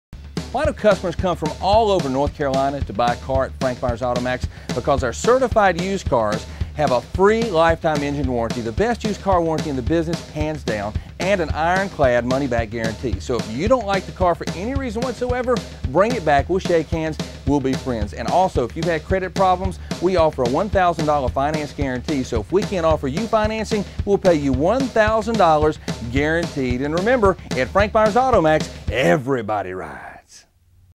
Frank Myers Auto Maxx - AutoTrader.com Vehicle Outro - Winston Salem, NC 27105
Uncle Frank & Tracy Myers are featured in this EXCLUSIVE AutoTrader.com Vehicle outro video for Frank Myers Auto Maxx in Winston-Salem, NC.

NOTE: This video is for entertainment value only. The offers contained in this video may not be valid.